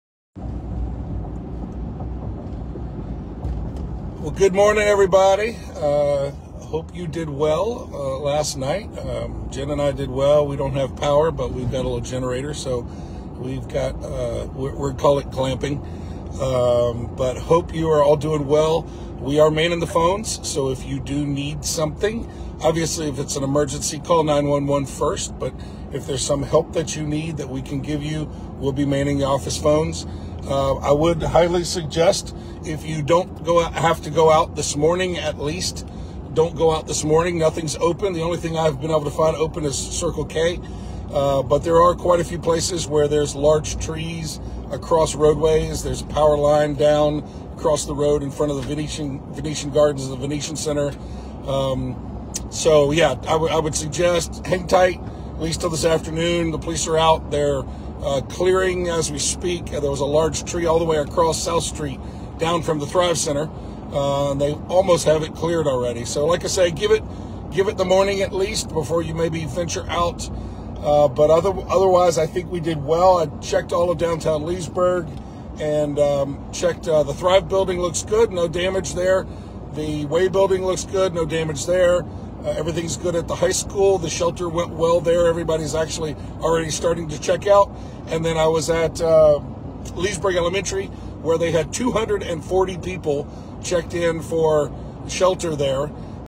10-10-2024 Hurricane Milton Update - Part 1
Church of The Lakes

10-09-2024 Hurricane Milton Update - Part 1

James 1:2-3 (NIV)
Consider it pure joy, my brothers and sisters, whenever you face trials of many kinds, because you know that the testing of your faith produces perseverance.